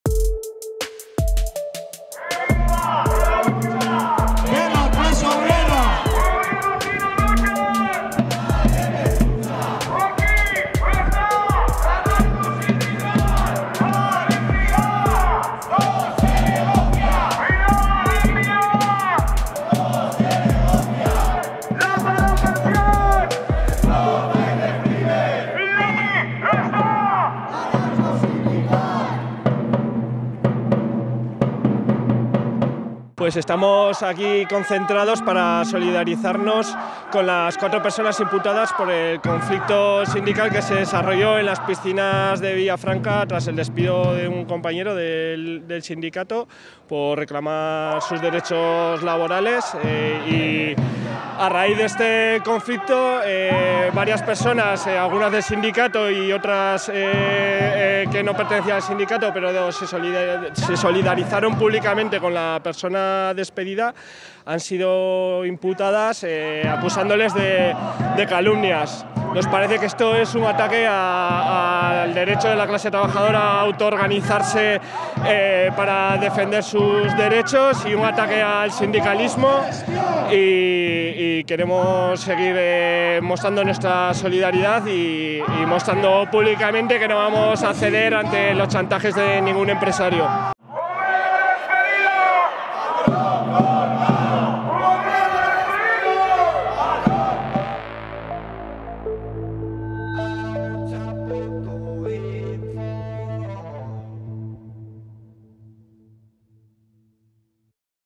CNT realiza una concentración en Iruñea en solidaridad con los imputados en Villafranca
El sindicato denuncia que cuatro personas han sido imputadas por Gestión y Eventos Lázaro por solidarizarse con un trabajador despedido en las piscinas de Villafranca en un juicio que tendrá lugar este lunes 18 en Tudela. CNT denuncia estas imputaciones como un ataque al derecho de la clase trabajadora a organizarse para defender sus derechos así como un ataque al sindicalismo y que no van a ceder ante los ataques de ningún empresario.